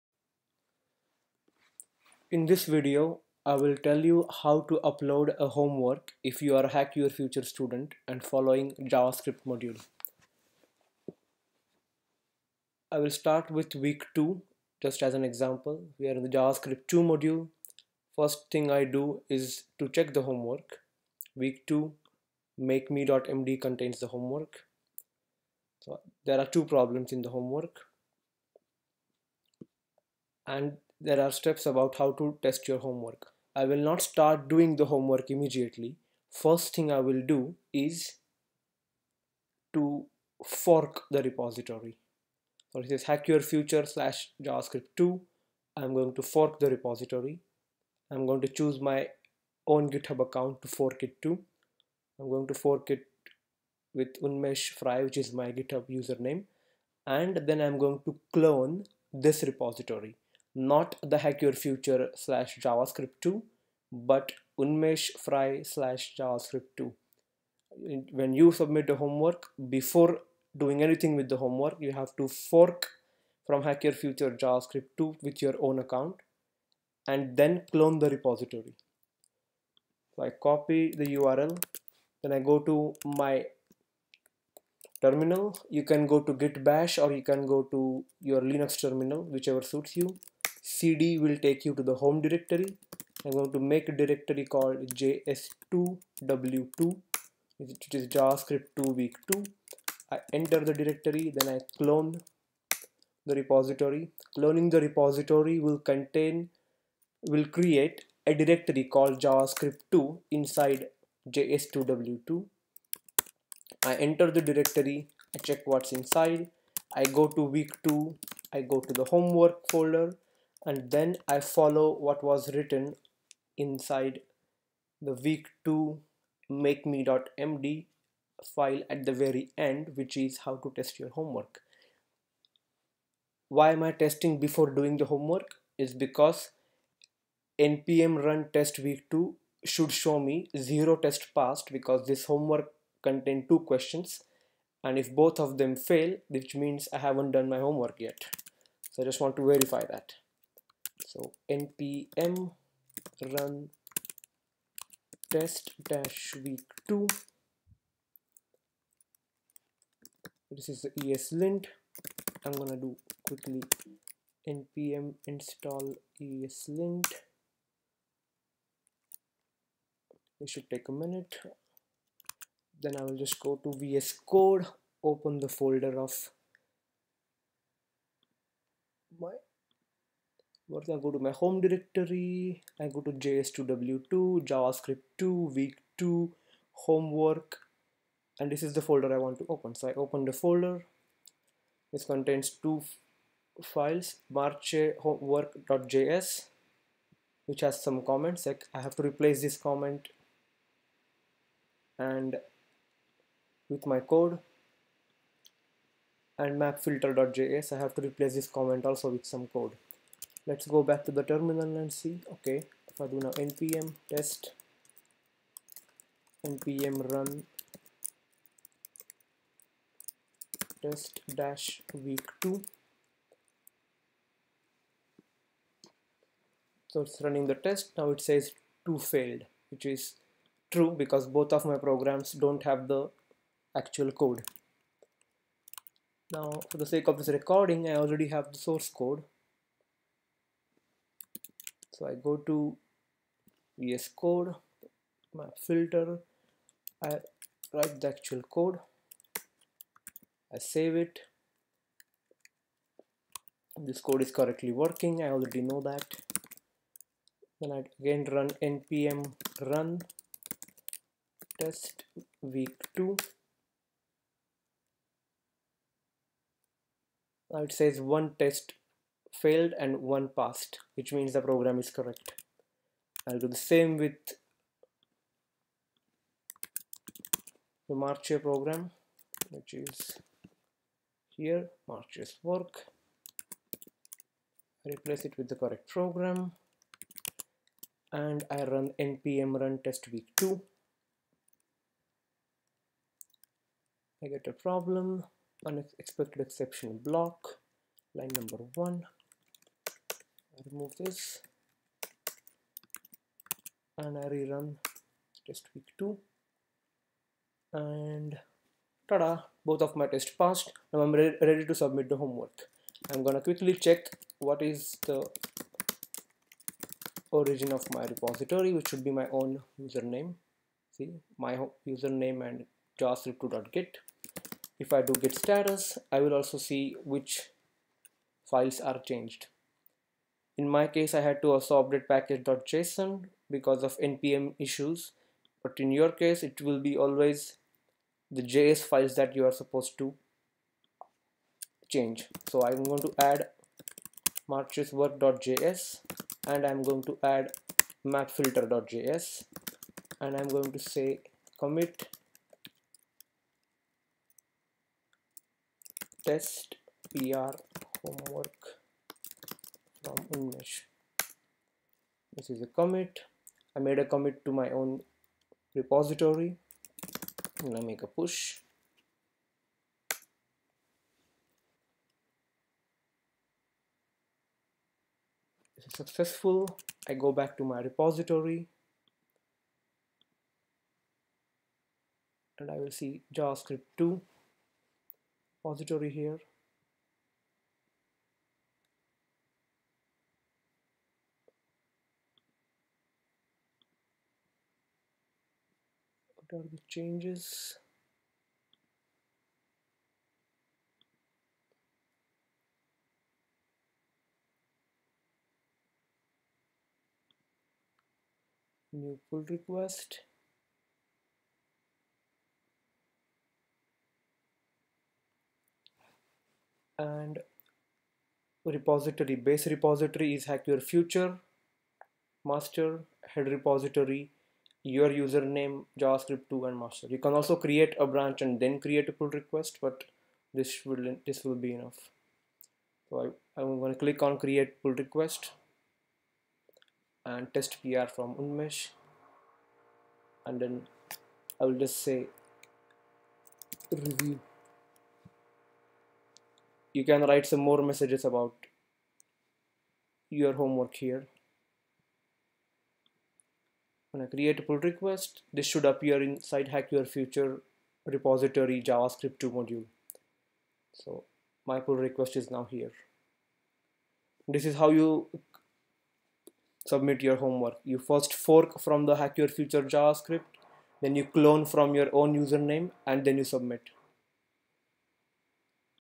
Submitting your Homework through Github
Step by step guide on how to submit your homework through Github